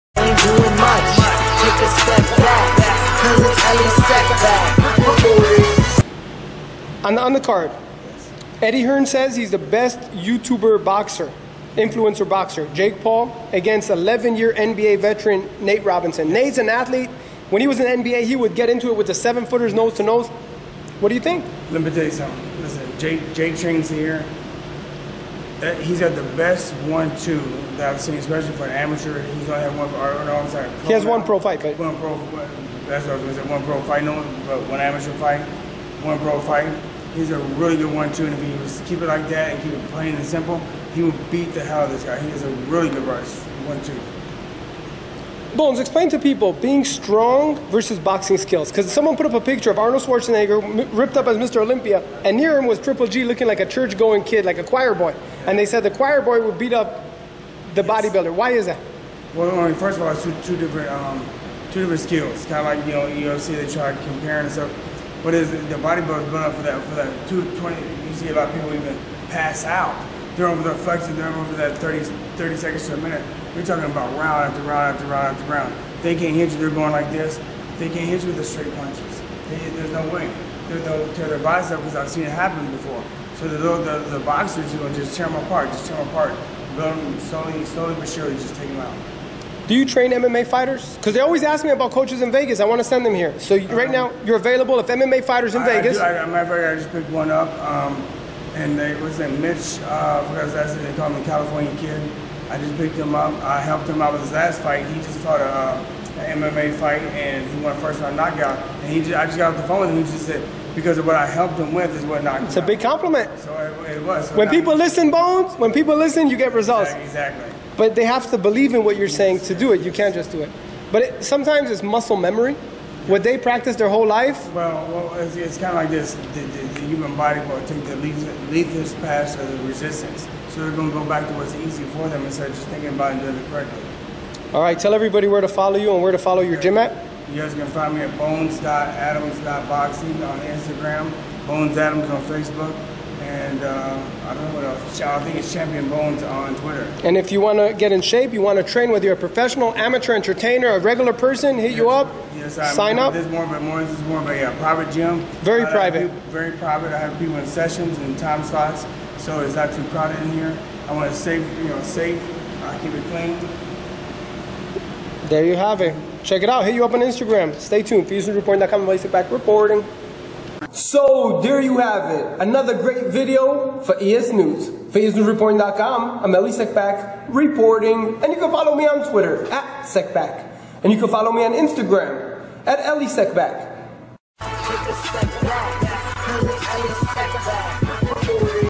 EDDIE HEARN Concerned with JAKE PAUL'S boxing skills| ESNEWS Boxing
Find out the latest discussion with Eddie Hearn and his thoughts on Jake Paul.

Eddie Hearn expresses his opinion about Jake Paul's training, sparring highlights, speed training, and fighting in the ring.

How will the YouTube Star do in his upcoming fight against Nate Robinson? ESNEWS has the latest news about Eddie Hearn, Jake Paul, Nate Robinson, and many others.